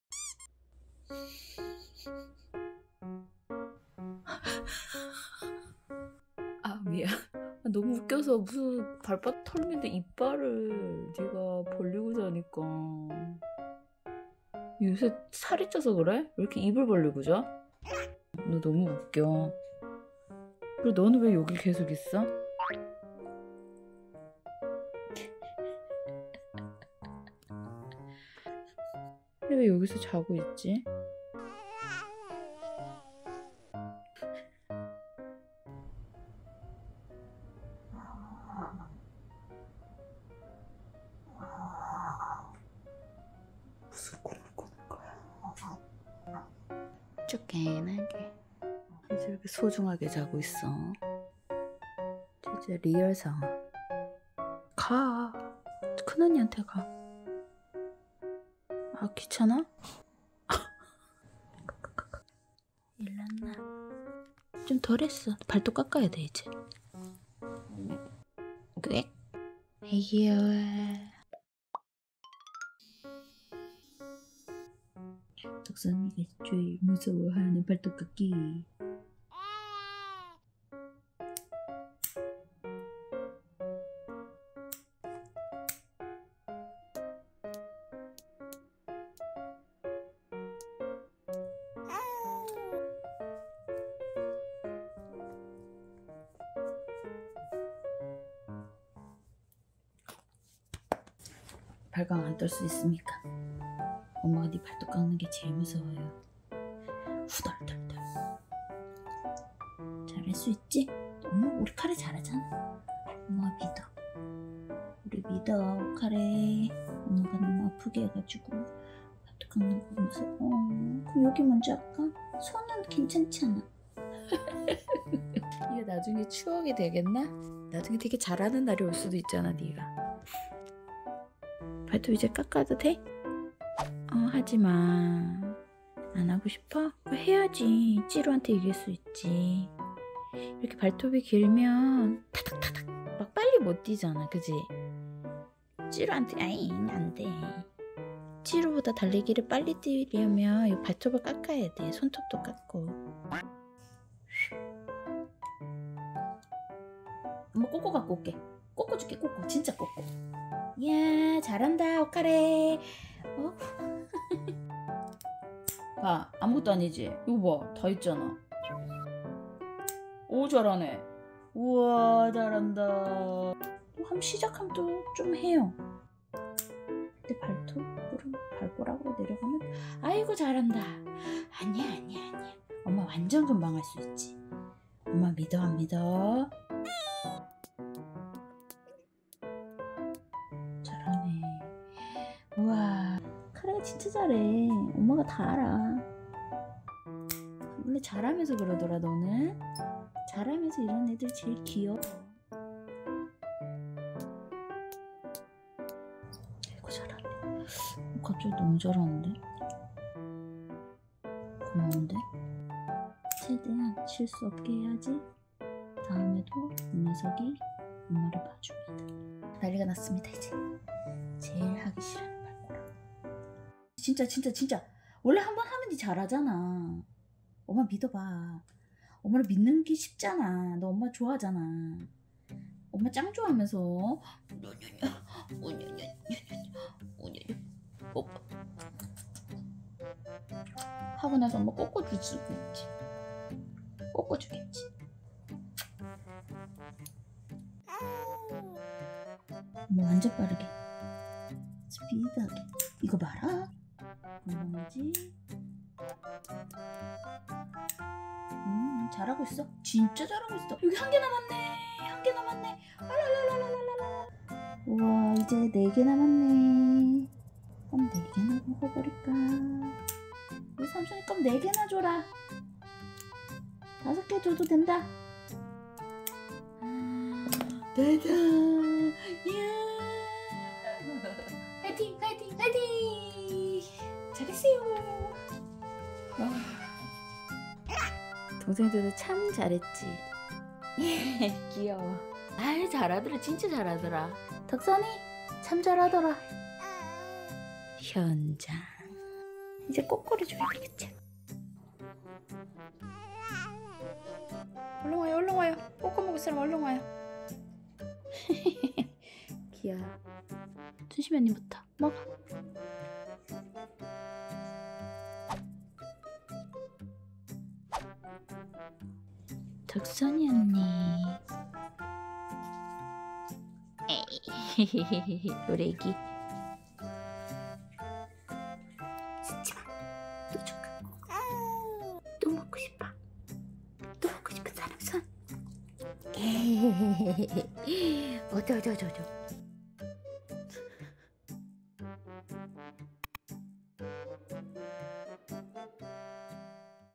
발톱 깎는 게 정말 싫다는 강아지 vs 발톱 깎다 입 벌리고 자는 강아지
순심, 덕선, 삼순의 일상 (Daily life of SunDeokSam)
■Instagram ID ☞ sun_sim_